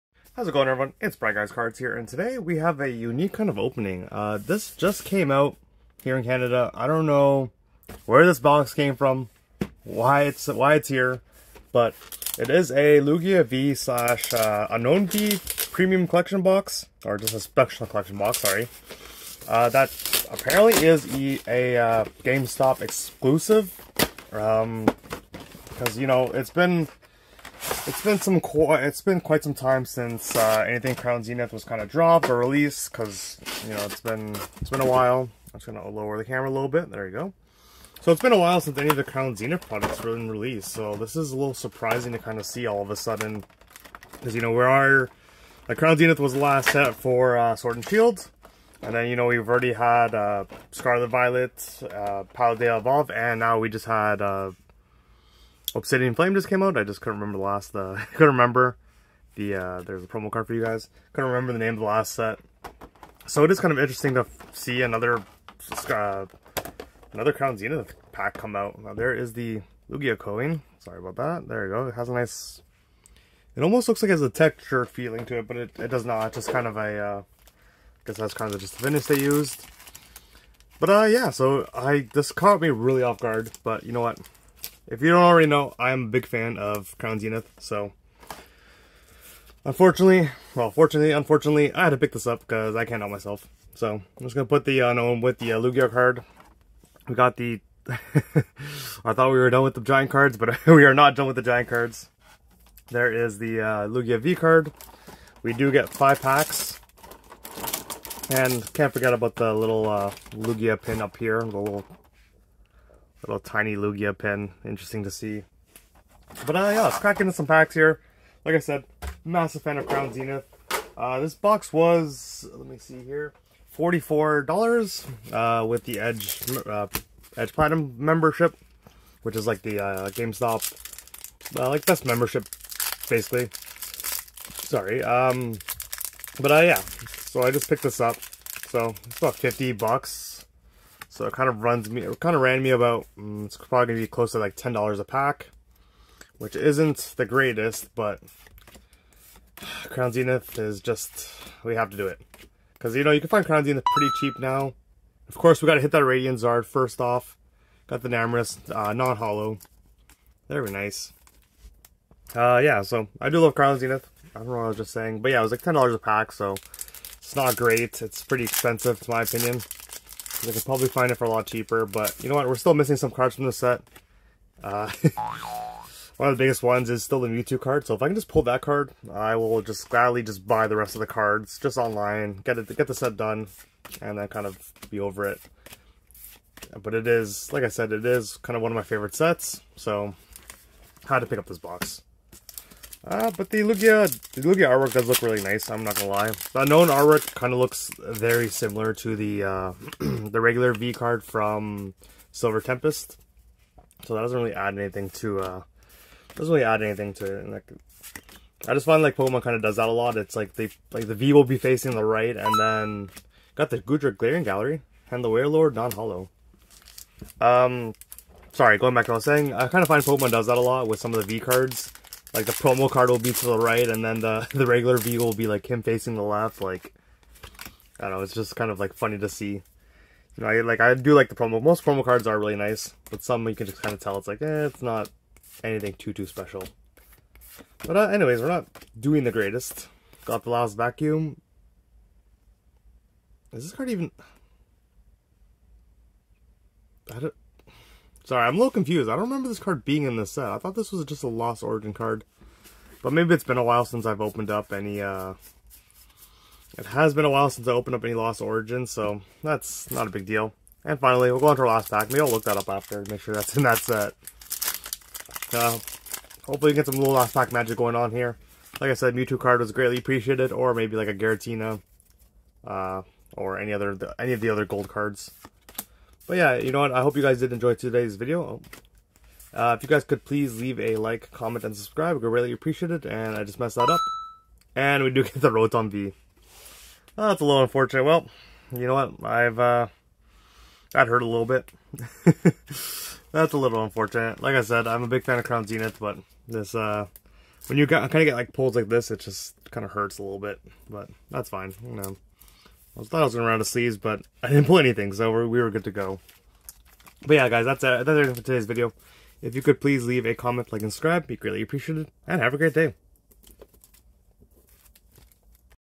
New Crown Zenith Product Is Still Being Released!? Pokemon Unown & Lugia Special Collection Opening
How's it going everyone it's Briguys Cards and today I'm opening up a whole new Pokemon Crown Zenith box! This box was just released and I honestly had no idea it was even coming out!